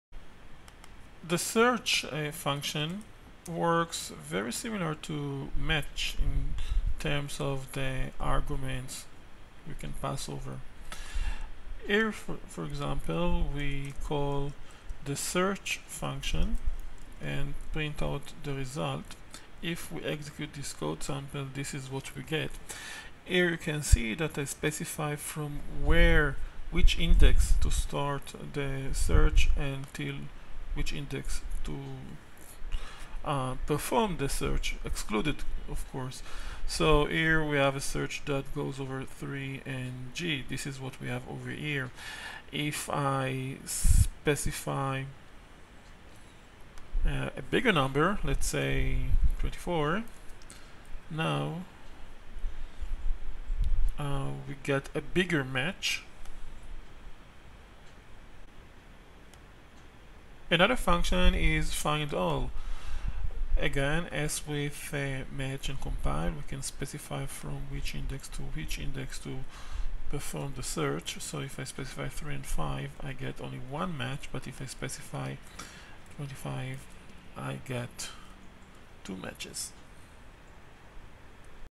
Python Regular Expression findall Function
When calling the findall function we can pass over, apart of the text been checked, the position from which we want the possible match to be checked and the position till where the check should last.